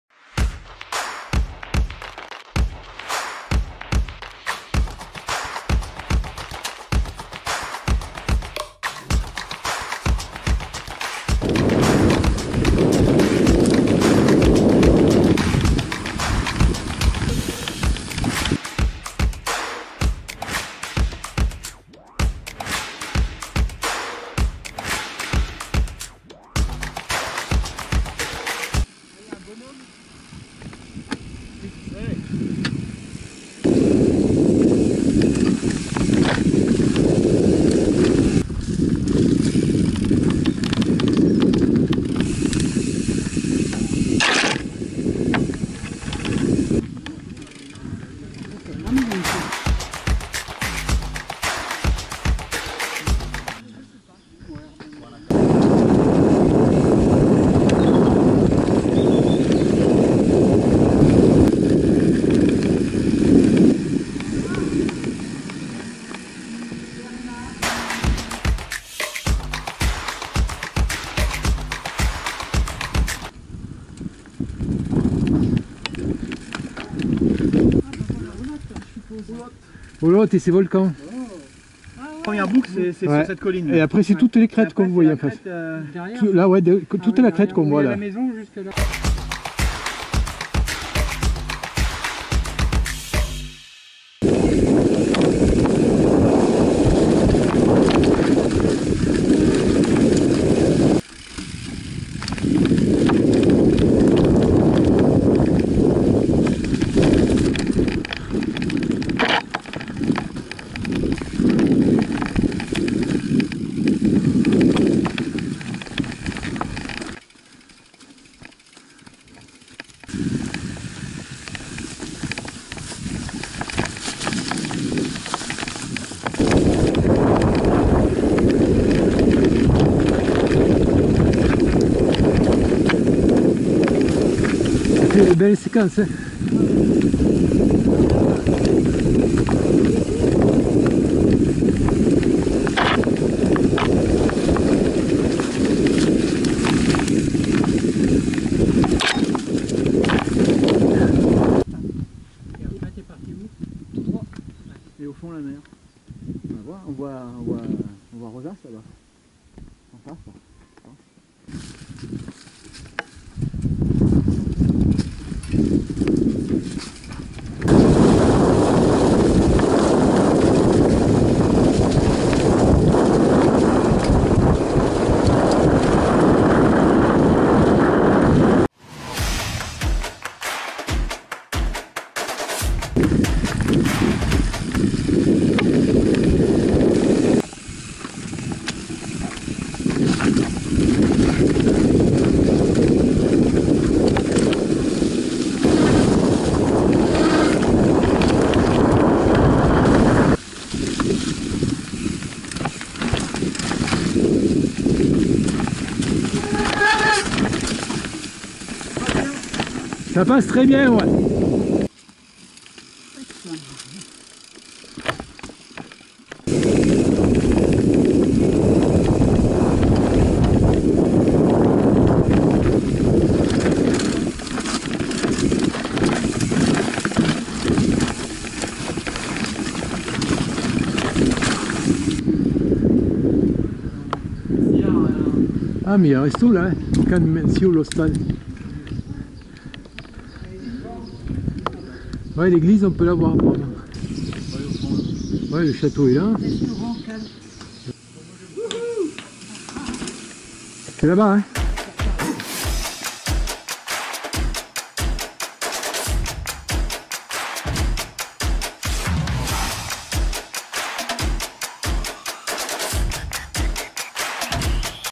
Zona Volcanica de Santa Pau relive
Viens avec nous dans la miam Zona Volcanica de Santa Pau ! 🤩😎
Le reportage et la trace gps et plein d'autres vidéos arrivent bientôt sur Miam Vtt